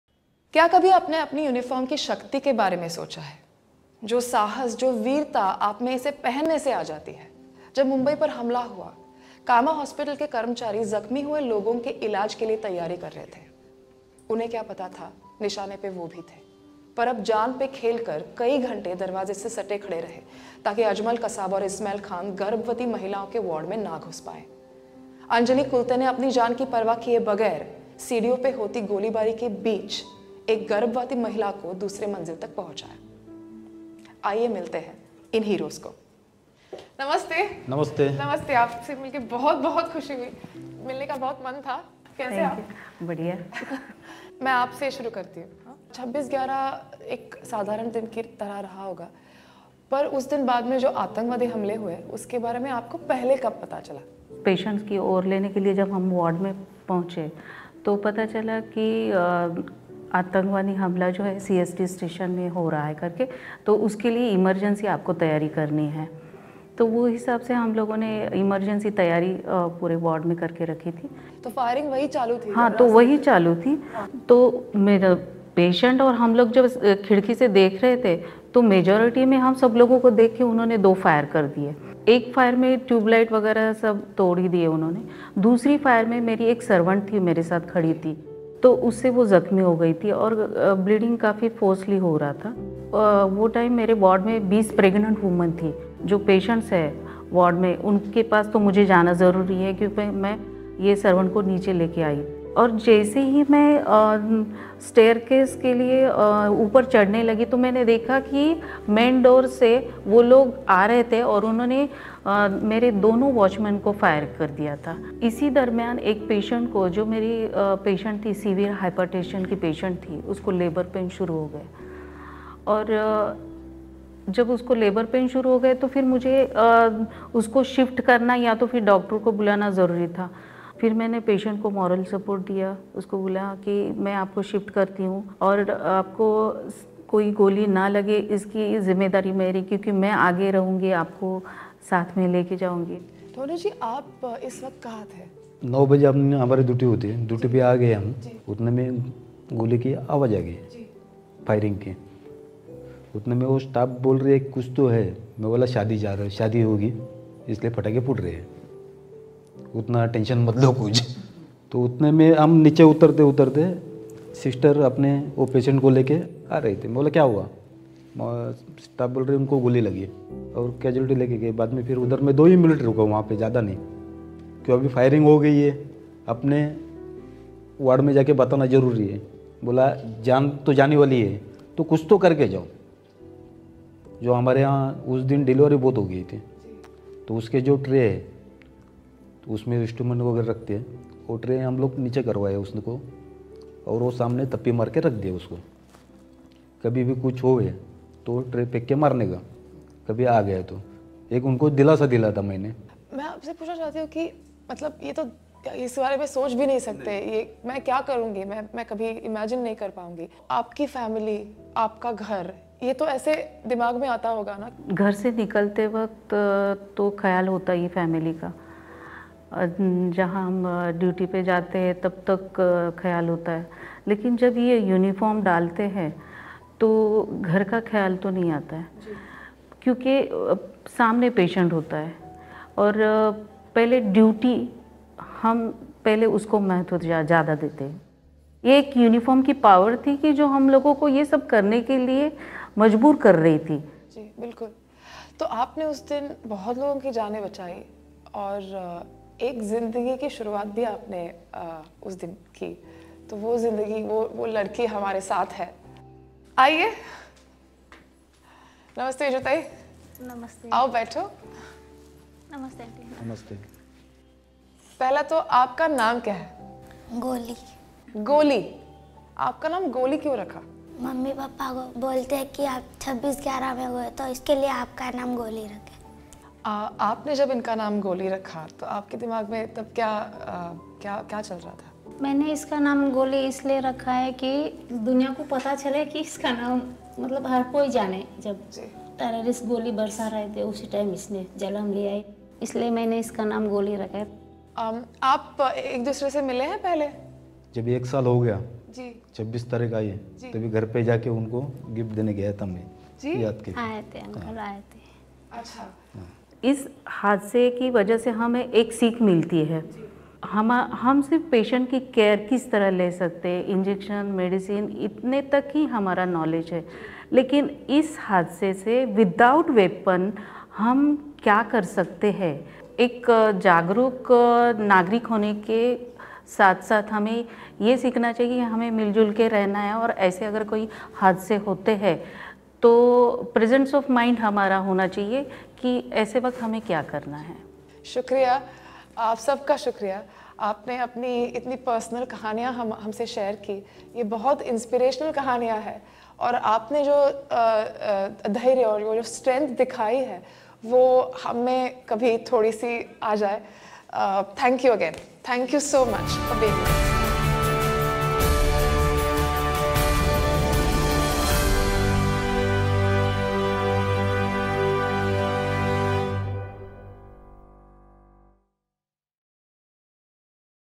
Radhika Apte meets the 'heroes' who helped pregnant women during the 26/11 attack.
Actor Radhika Apte talks about the importance of uniform and speaks to the 'heroes' who helped pregnant women in labour during the 26/11 attack.  "It was the power of the uniform that made us save and help the people," says the nurse who took care of her patients on a fateful day.